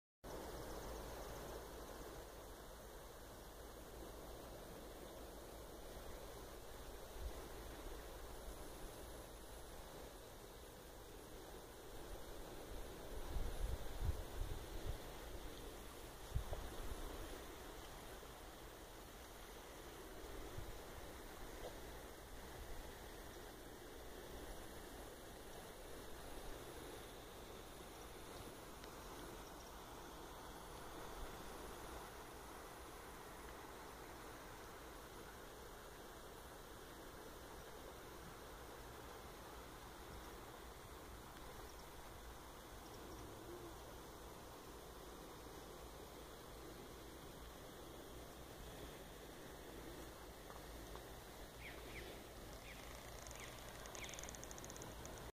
Views inbetween Cape Cornwall and Sennen Land's End
What incredible glorious day today.
I take a walk from Cape Cornwall to Sennen.